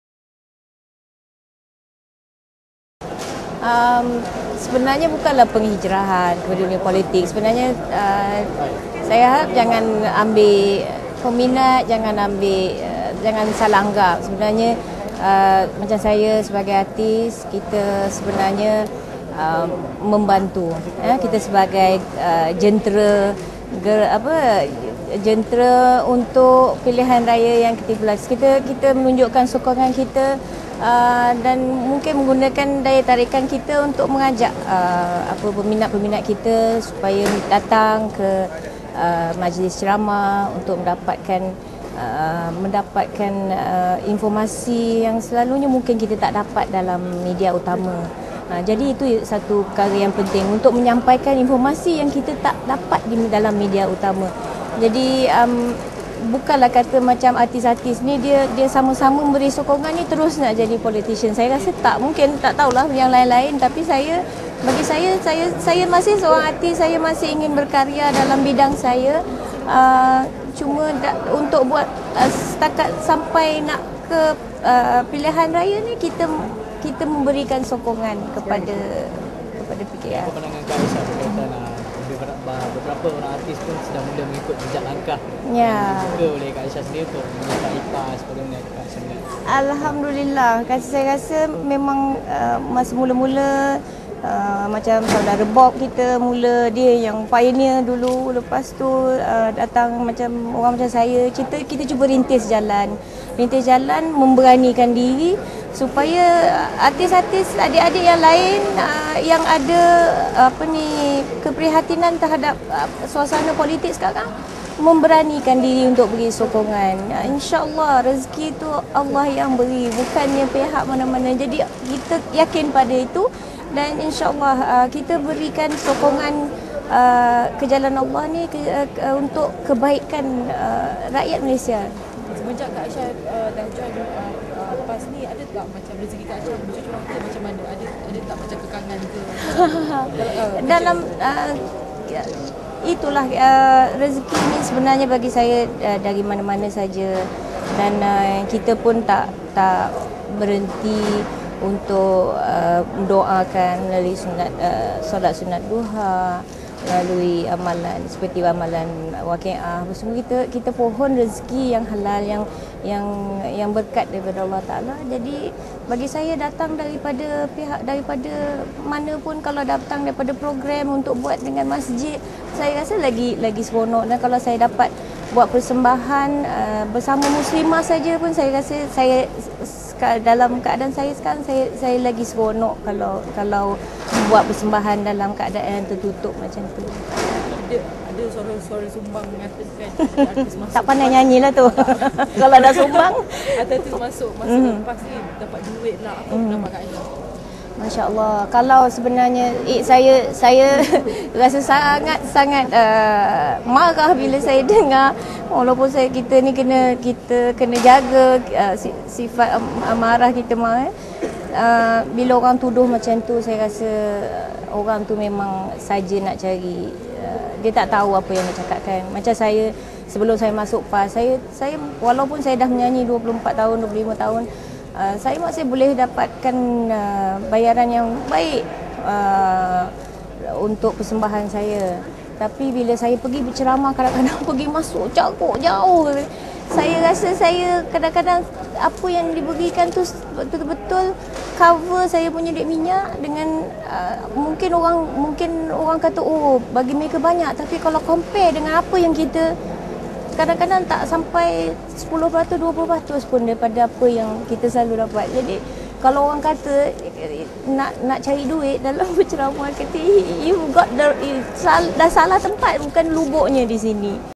Bayaran: Penjelasan Aishah
Penyanyi Aishah memberi penjelasan berhubung bayaran di atas undangan memenuhi majlis yang melibatkan beliau. Ikuti penjelasan beliau pada majlis ceramah beliau di Dewan Datuk Fadzil Mohd Noor, Taman Melewar pada 07 Julai 2012.